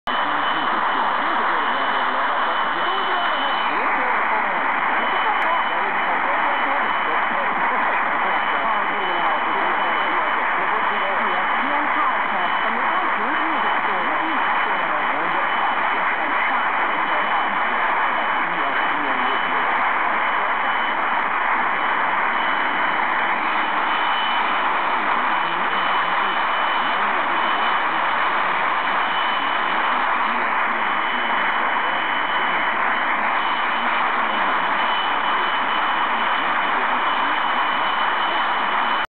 MW-DX: ESPN 1470 Lewiston, ME in Geeste, Germany - 5.526km
QTH: Geeste/Emsland, Germany (52°36'N/7°16'E)
RX: Lextronix E5 + ferrit antenna

ESPN
on AM 1.470 kHz from Lewiston, Maine (USA)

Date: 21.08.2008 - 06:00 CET
Nice IDs:
- "ESPN-Sportstalk" (00:12s)
- "ESPN-Radio" (00:19s) at top of the hour !

distance Lewiston - Geeste: 5.526km

No FM or TV-DX cdx that day, but a nice transatlantic opening on mediumwave towards North America made this reception possible.